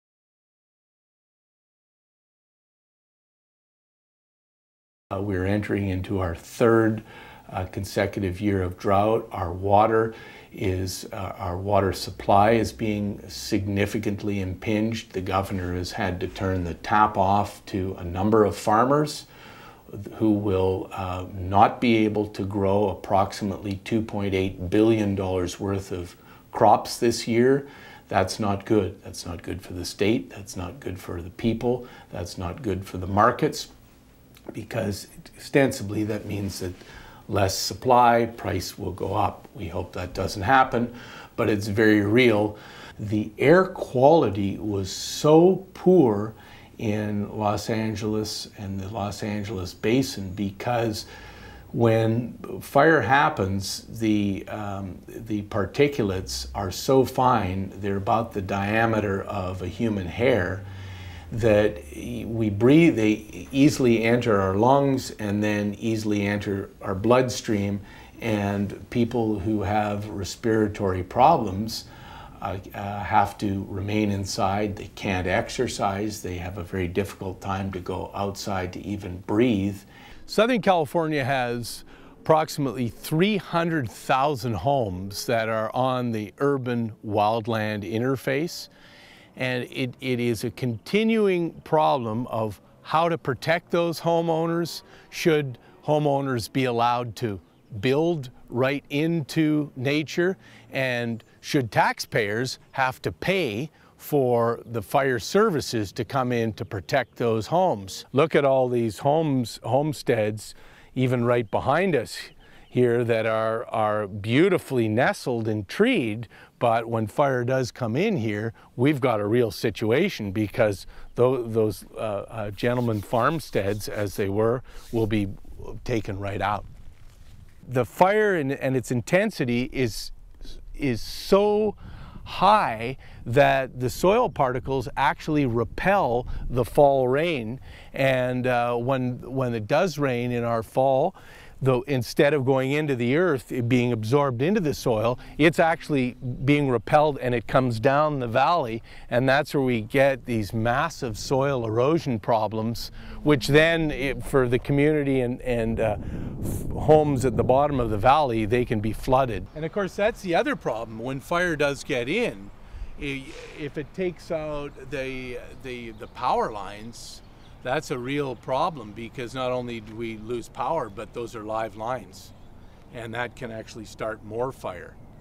The Effects of Wildfires
Learn about the effects due to wildfires in California. An interview with renowned fire expert Dr. Reese Halter.